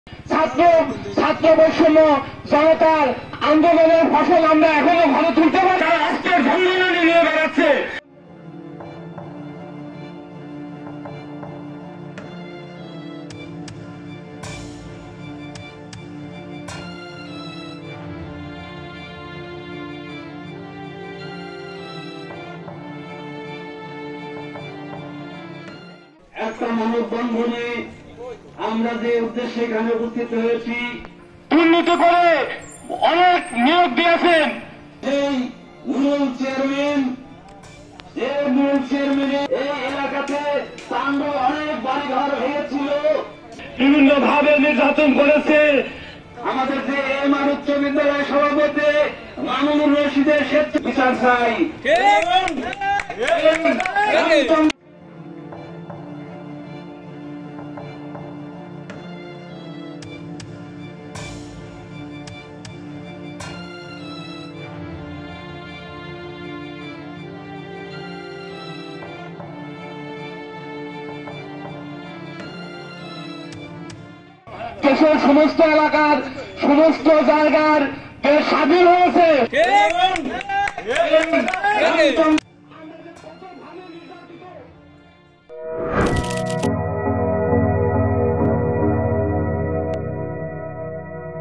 পাবনার চাটমোহরে আওয়ামী লীগ নেতার বিচারের দাবিতে মানববন্ধন | News | Ajker Patrika
পাবনার চাটমোহর উপজেলার গুনাইগাছা ইউনিয়ন আওয়ামী লীগের সভাপতি ও সাবেক ইউপি চেয়ারম্যান নুরুল ইসলামের অনিয়ম-দুর্নীতির বিচারের দাবিতে ২ সেপ্টেম্বর মানববন্ধন করেছেন এলাকাবাসী। বিস্তারিত ভিডিওতে।